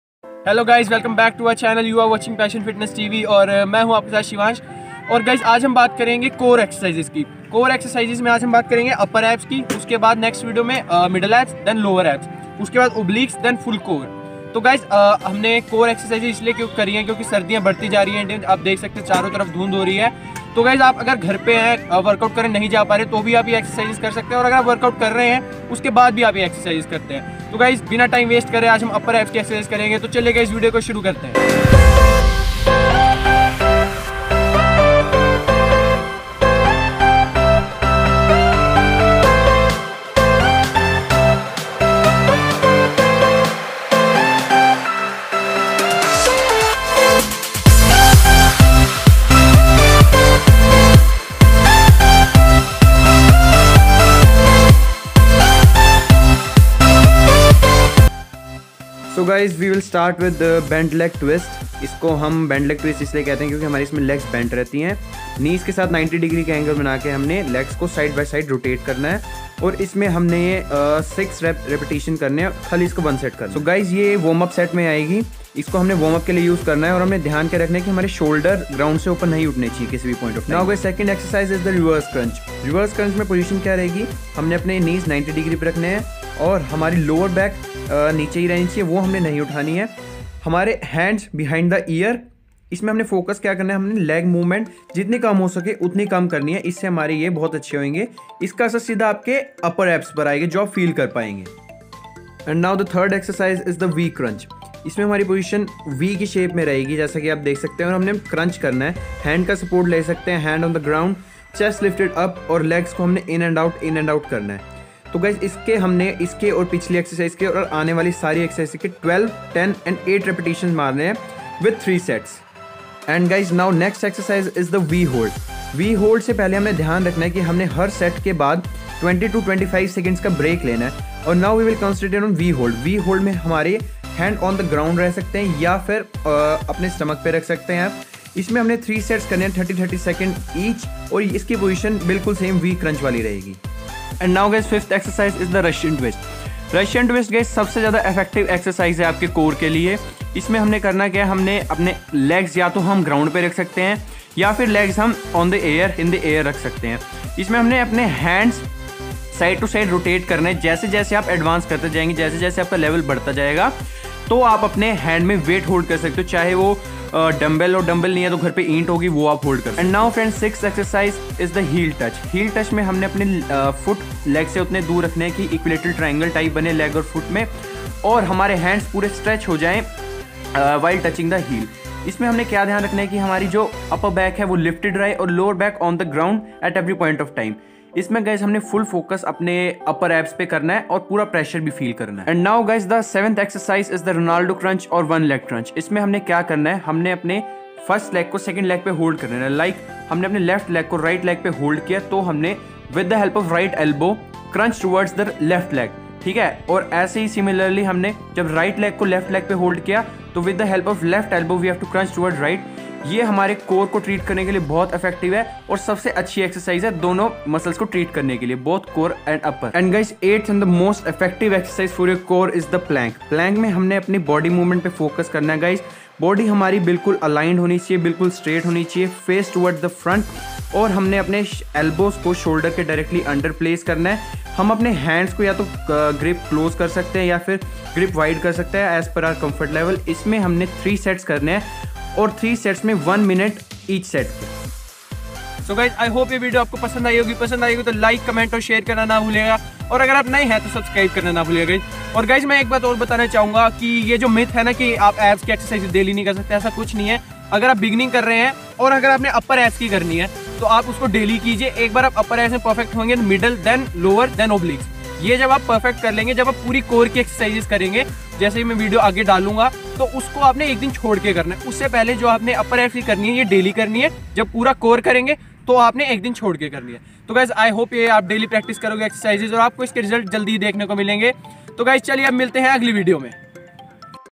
CORE WORKOUT|Wajan kam nhi hota to ye dekhe (100%working) Hindi tutorial part-1| Passion Fitness Tv
Passion Fitness Tv

In this video we talk about how to strengthen core and how to lose belly fat. we made a bunch of 8 exercises that helps to strengthen core and also cut fat. various exercises involved and reps and sets are as follows

1. Bent leg twist 1 Set, 6 Reps
2. Reverse crunches 3 Sets and 12,10 and 8 reps
3. V-crunch 3 Sets and 12,10 and 8 reps
4. V-hold 3 sets 30 sec hold
5. Russian twist 3 sets  and 12,10 and 8 reps
6. Heel touch 3 Sets and 12,10 and 8 reps
7.Ronaldo crunch 3 Sets and 12,10 and 8 reps
8. Plank 3 sets and 1 min hold in each set

Do this workout daily. strengthen your upper abs